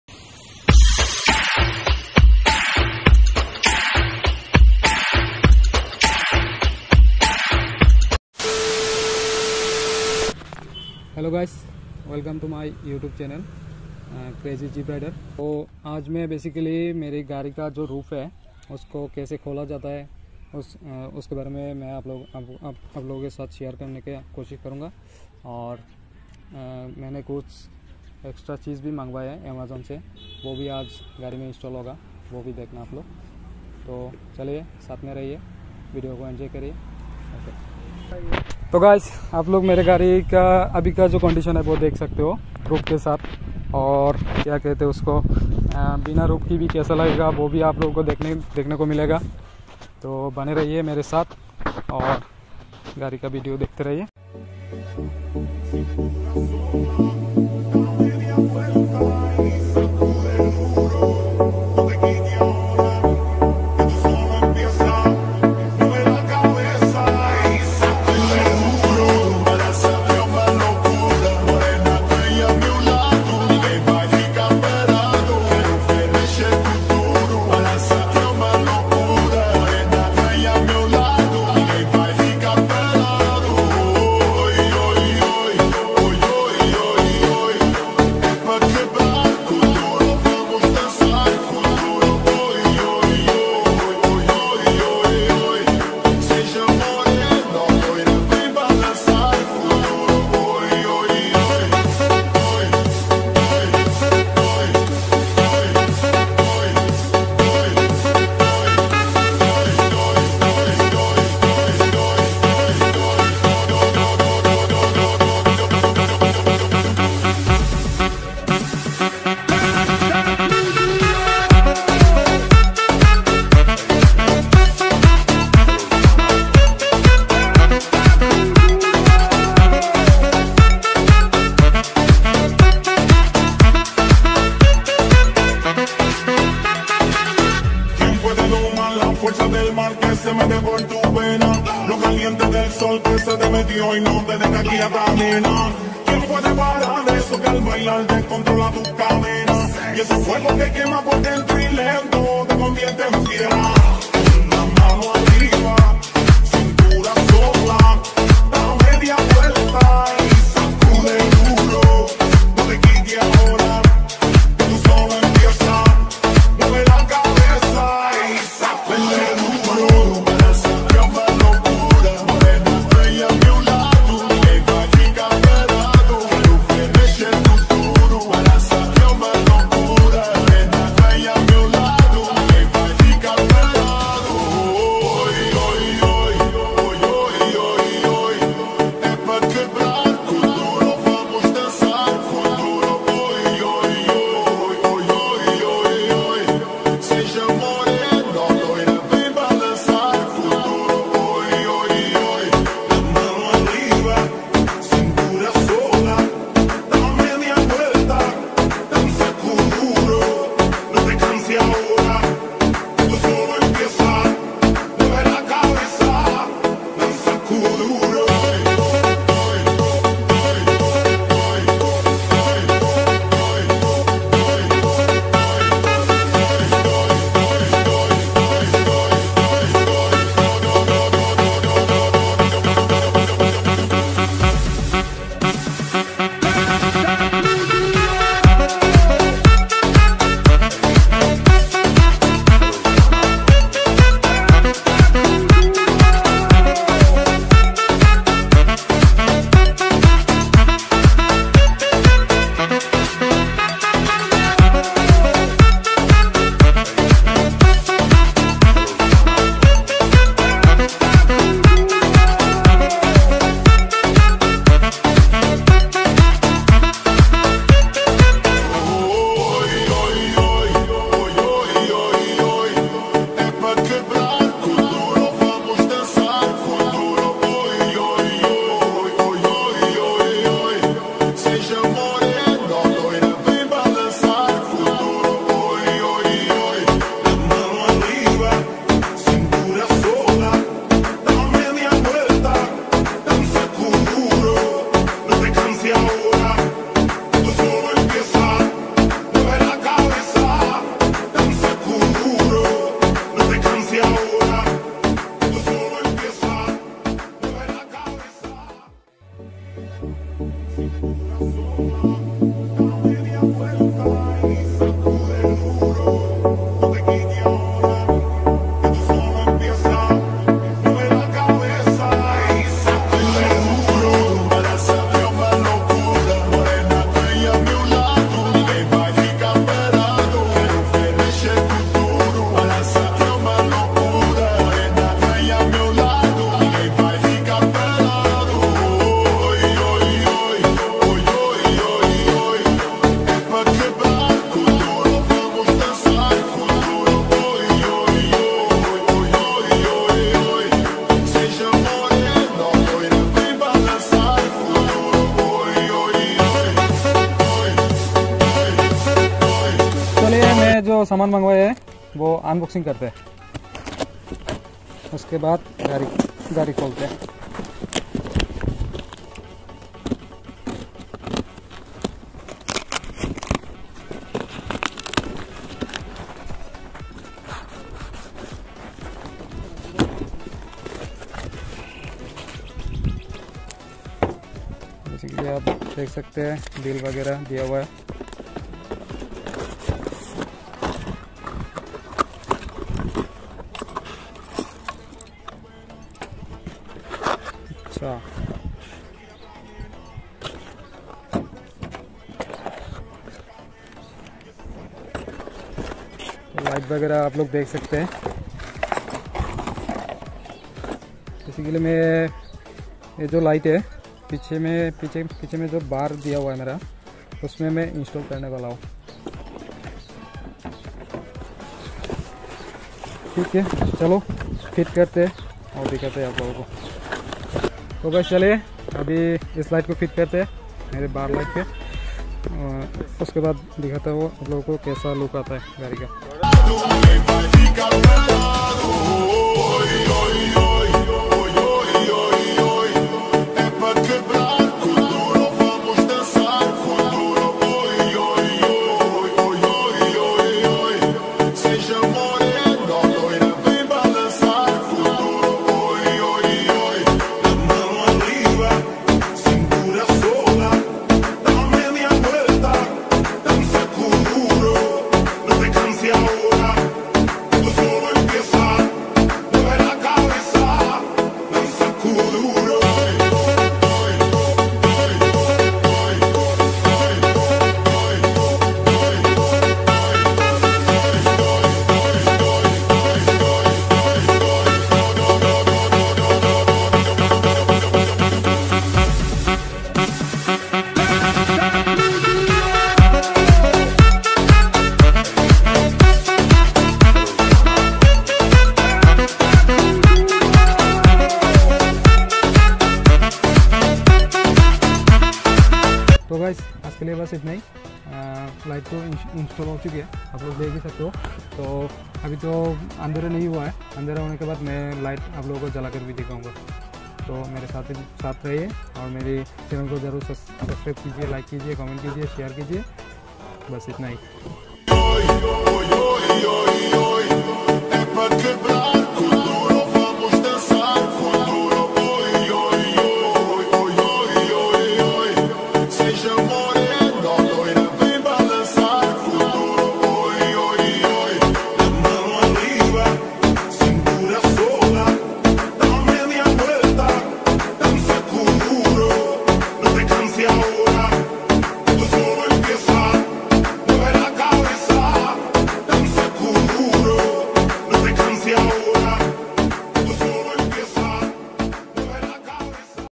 POORA GAADI KHOL DIYA🥴 || FIRST CUSTOM MADE JEEP IN KHOWAI || Crazy Jeep Rider
Aaj gaadi kaa poora roof khol k nanga kar diya, phir naya bar light install kya. aap video dekhiye aur maja leeniye. this video is only for entertainment, please like comment share and subscribe.